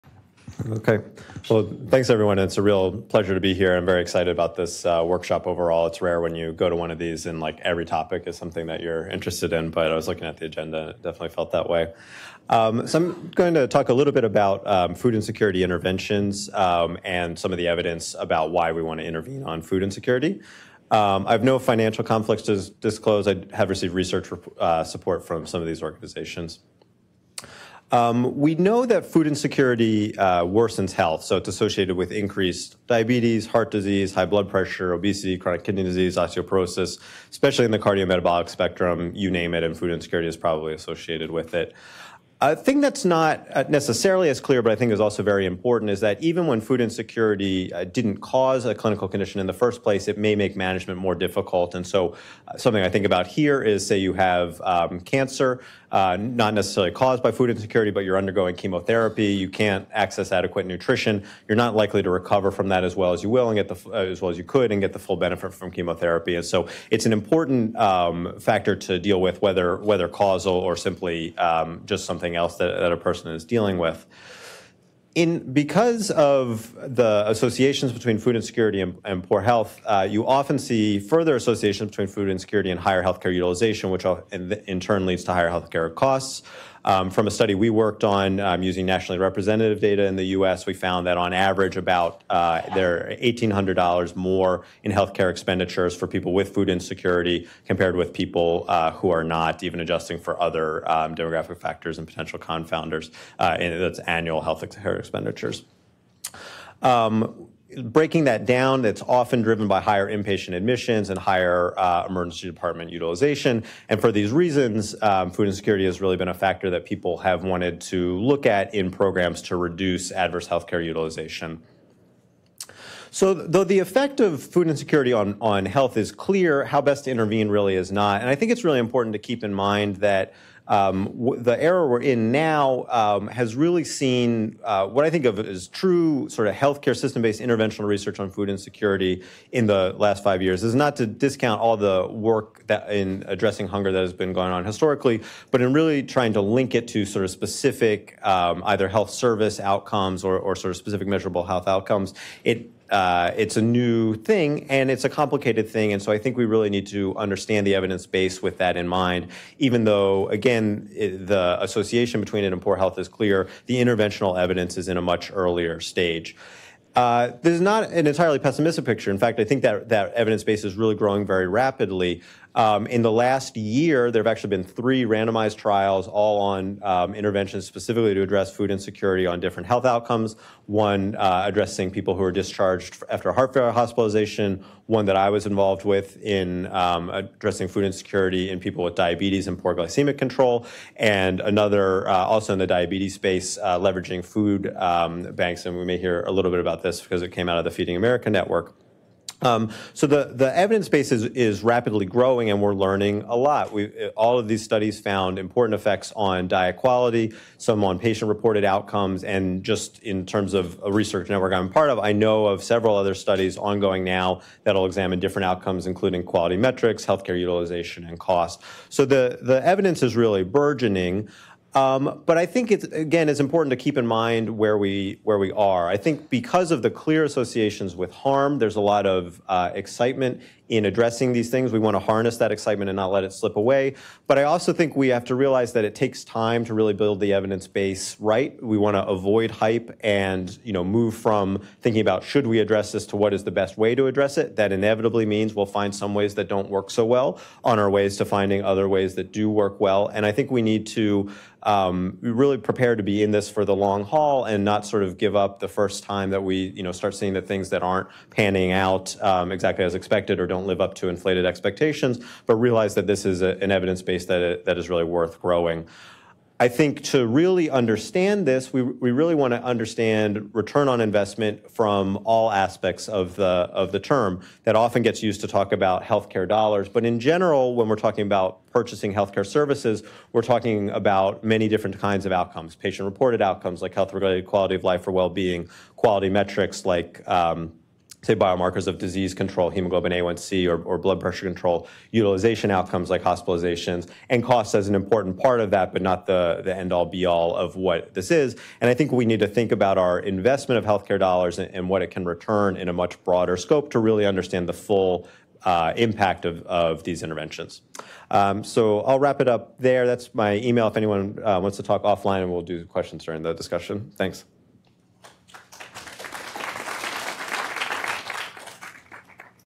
4/26/2019 - Panel 3: Berkowitz
Seth Berkowitz, M.D.
Assistant Professor of Medicine, Division of General Medicine and Clinical Epidemiology, Department of Medicine, School of Medicine, University of North Carolina at Chapel Hill